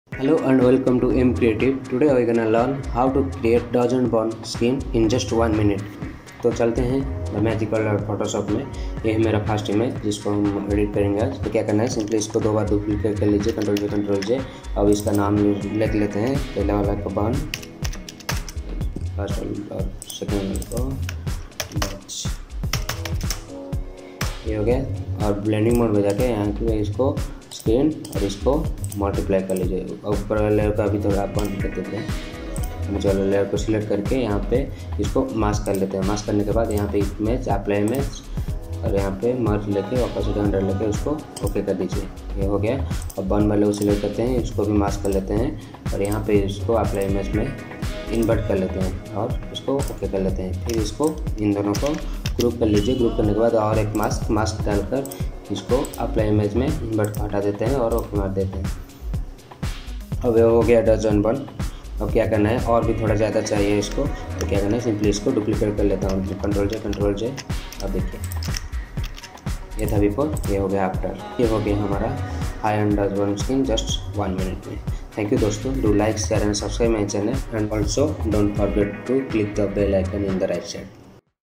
DODGE & BURN Skin Retouching with Just 2 Layers || PHOTOSHOP TUTORIAL
just use of two layer copy and apply image for brighter and dark part to create a smooth dodge and burn skintone.